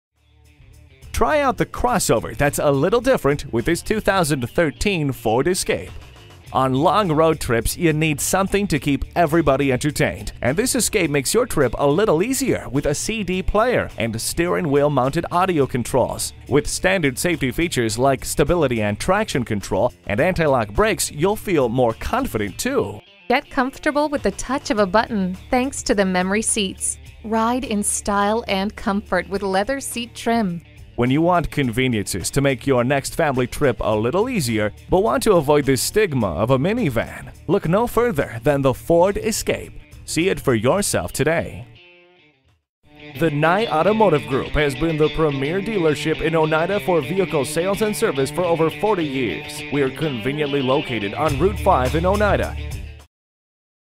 2013 Ford Escape Utica NY Oneida, NY FR5633A - SOLD
Year: 2013
Make: Ford
Model: Escape
Trim: SEL
Engine: 4 Cyl. 1.6L
Transmission: Automatic
Color: Tuxedo Black
Mileage: 72749
Stock #: FR5633A
VIN: 1FMCU9HX5DUA23551
This 2013 Ford Escape SEL is located at Nye Ford in Oneida New York.

Address:
1441 Genesee St.